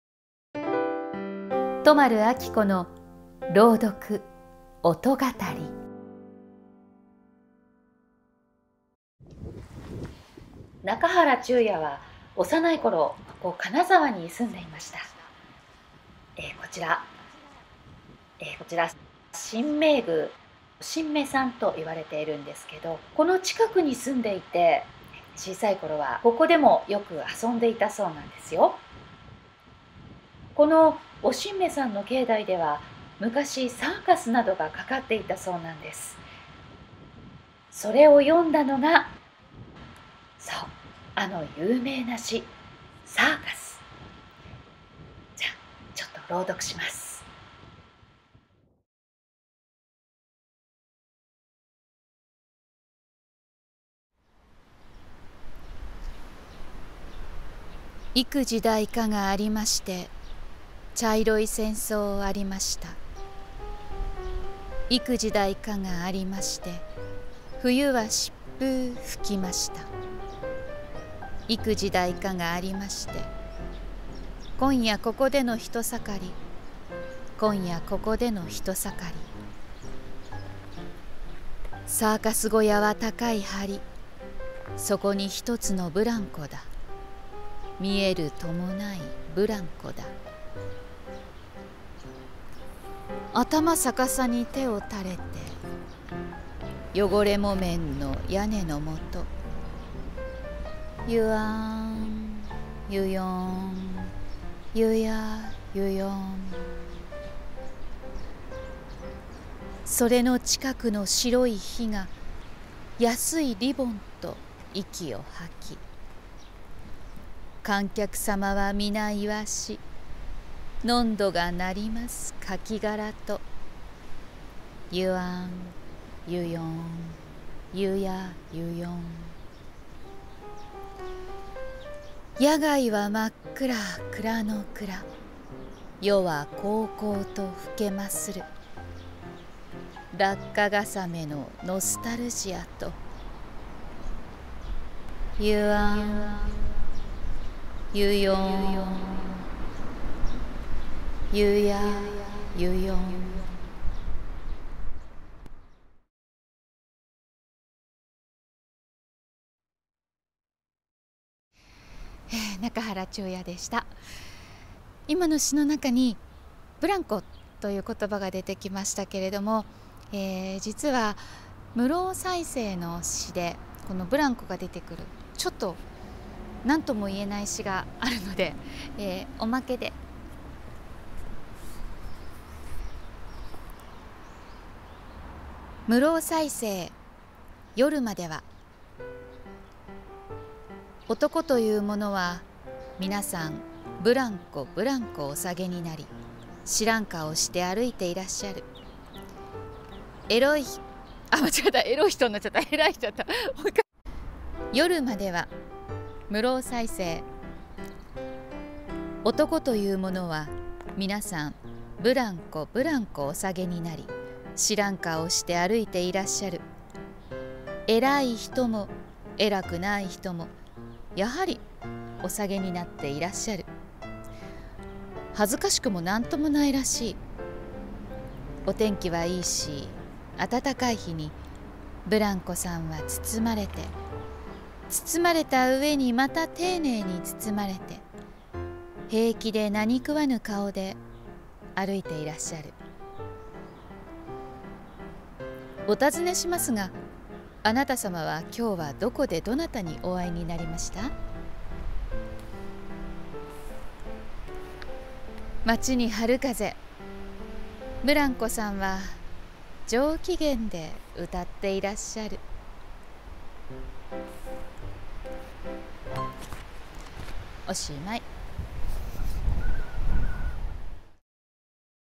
戸丸彰子の朗読音がたり「中原中也」
このビデオは アナウンサーで朗読家の戸丸彰子が語る「朗読音がたり」です。今回は中原中也の詩を金沢の風景とともに語ります